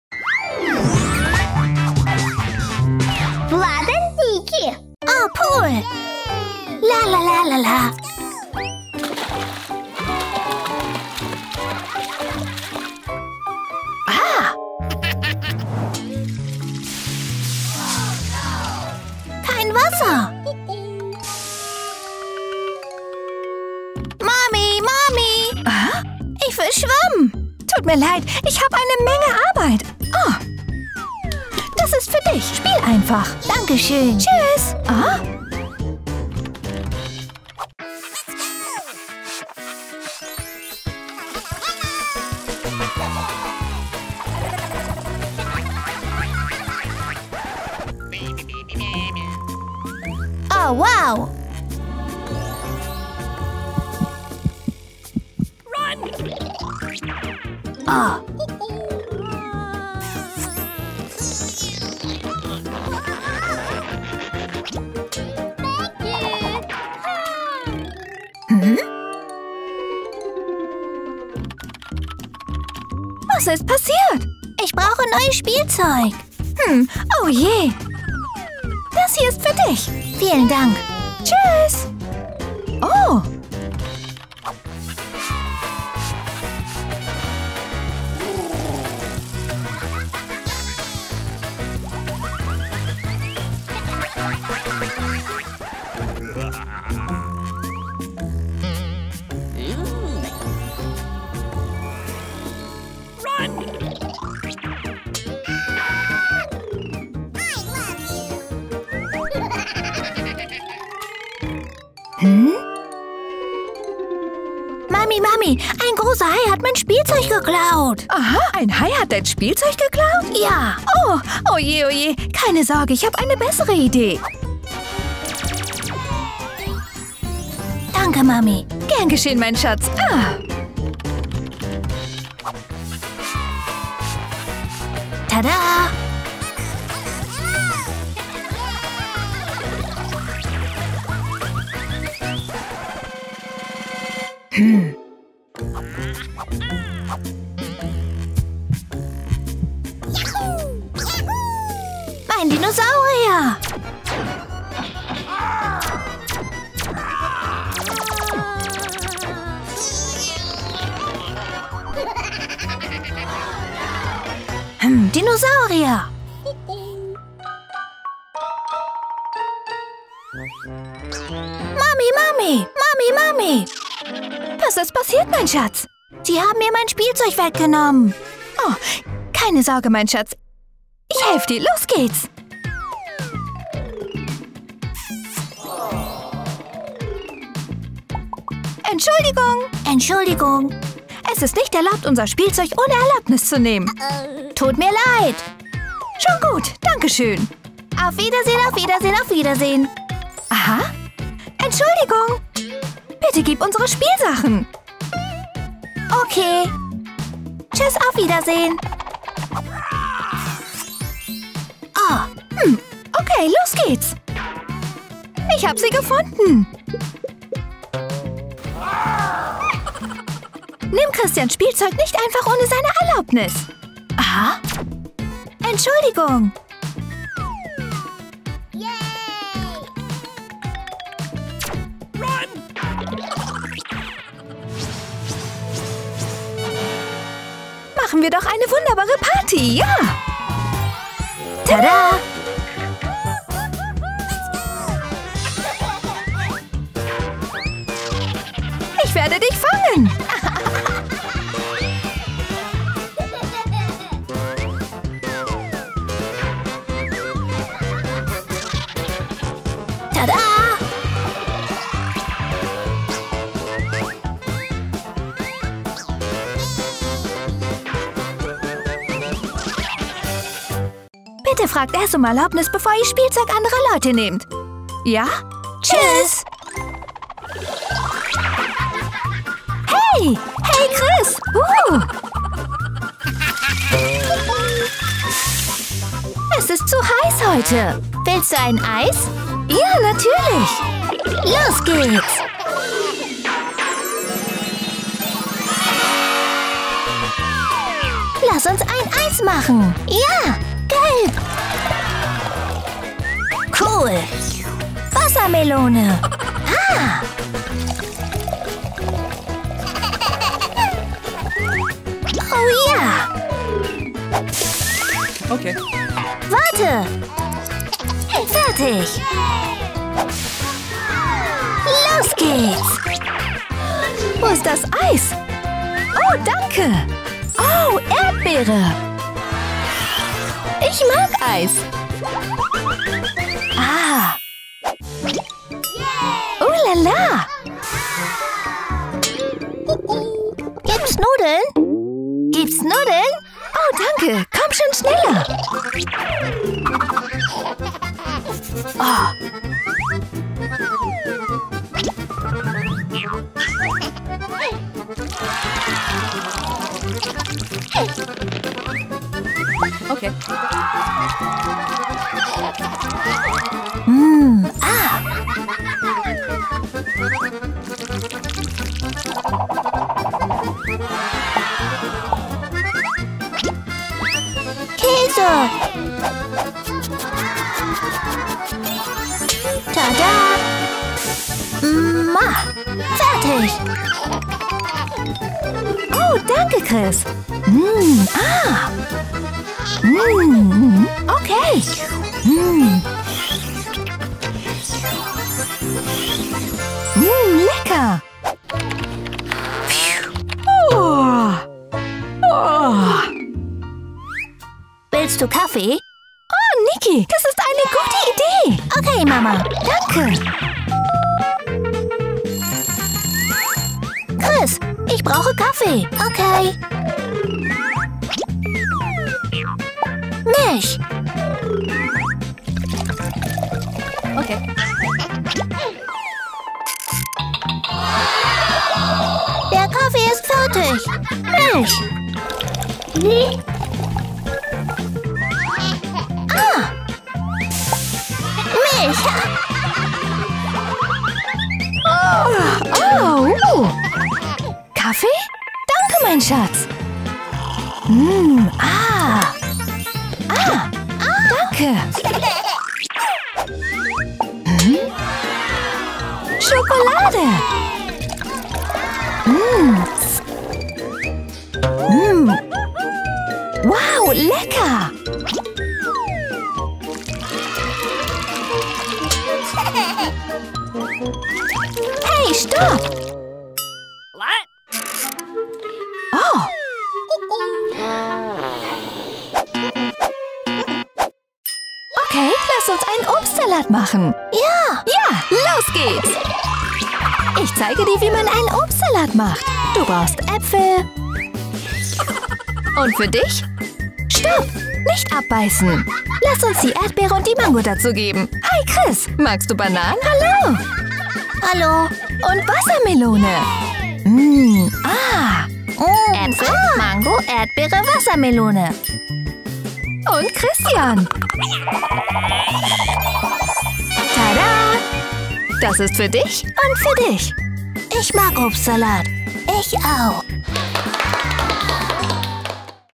Chris und Mama lernen, aufblasbare Spielzeuge zu teilen
00:00 Lernen Sie, aufblasbares Spielzeug zu teilen
04:38 Niki und Chris lernen, für Mama zu kochen